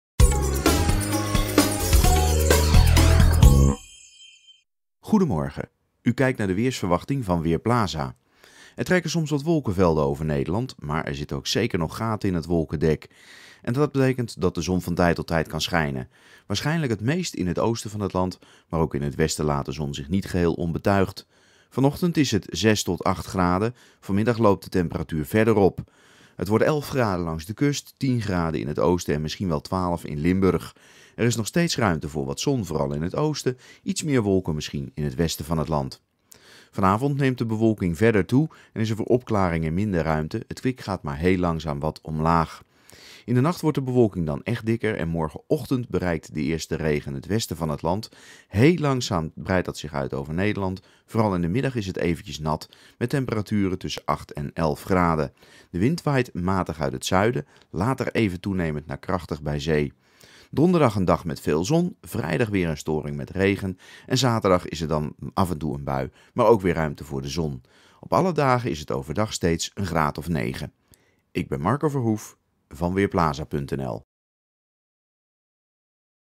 Weerbericht dinsdagochtend: Soms zon
De zon schijnt af en toe, maar er zijn ook wolkenvelden. Het blijft droog en de temperatuur ligt vrij hoog. Het wordt 10 tot 12 graden. De wind waait matig tot vrij krachtig, kracht 3 tot 5, uit een zuidelijke richting. Ook komende nacht is het droog, met enkele opklaringen. Het koelt af tot tussen 3 en 5 graden. Morgen is het een graad of 9. De bewolking neemt toe en in de middag trekt er wat regen of motregen over het land. Bij zee kan de wind even krachtig zijn, kracht 6.